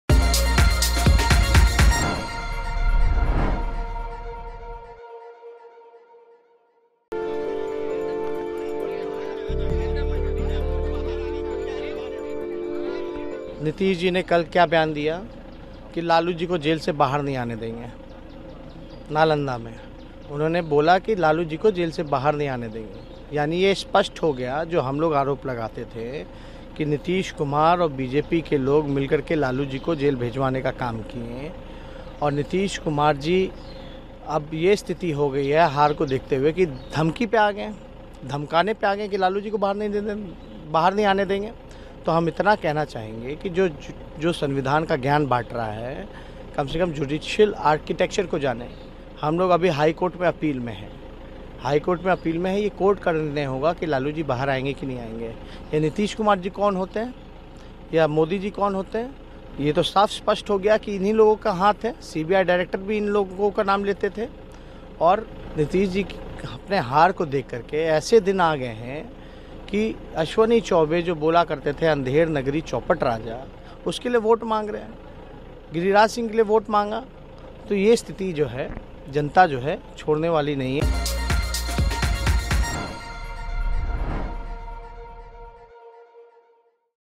BJP और Nitish Kumar पर Lalu को लेकर Tejashwi का बड़ा आरोप
आरजेडी नेता और लालू यादव के बेटे तेजस्वी यादव ने नीतीश कुमार और बीजेपी पर बड़ा आरोप लगाया है.